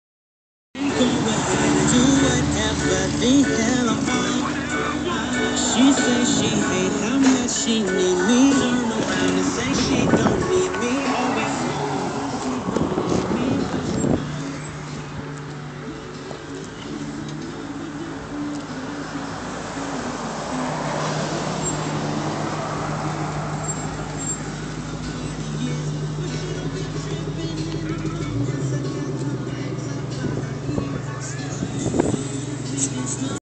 VIP Lexus LS400 Cruising / Close-Up shots
My boy's '92 LS400 Flowmasters Rota 18x9.5 +20 super fitted whip pulling in my driveway to chill. Super clean.